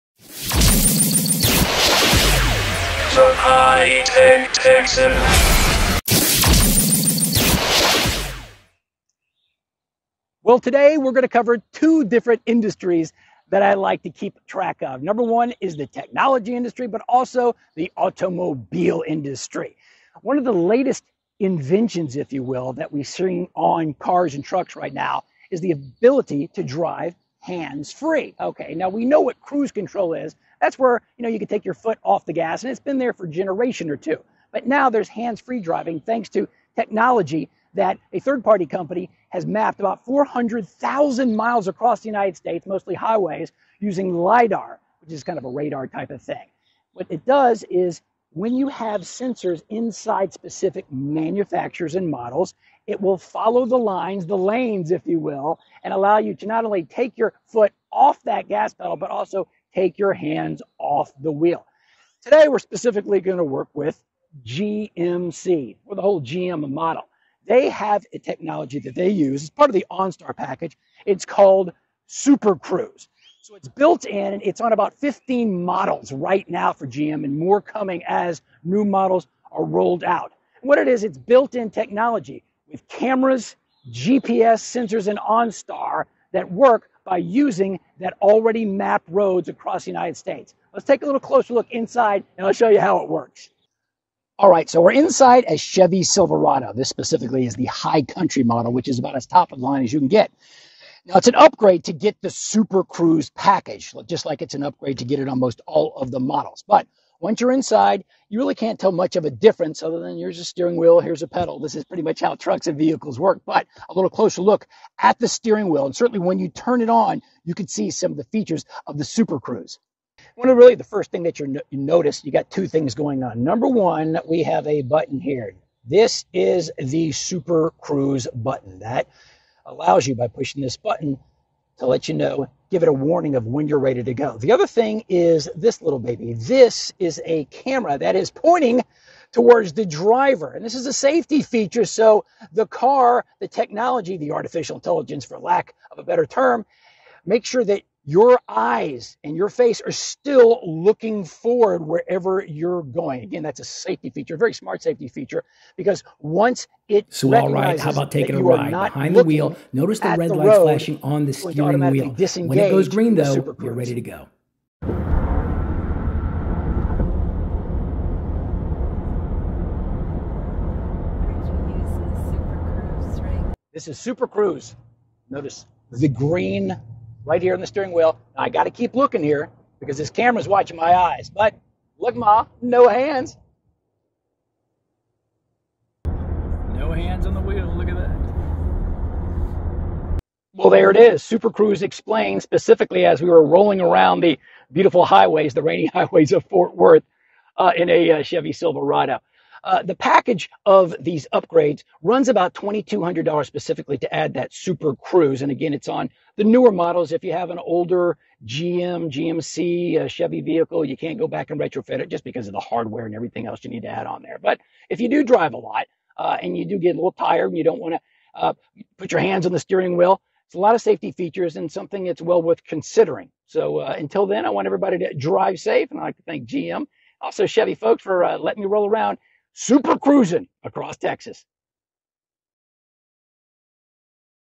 How "Super Cruise" Lets You Drive Hands-Free
We all know what "cruise control" is and that has been a driver's convenience for decades allowing you to take your foot off the gas pedal to travel at a constant speed.

Technology now allows for much more with the advent of on-board cameras, LIDAR, GPS, and more. GM offers Super Cruise on many of its new fleet to not only let drivers take their foot off the pedal but also their hands off the wheel.

Here is a quick overview of how it works on a Chevy Silverado.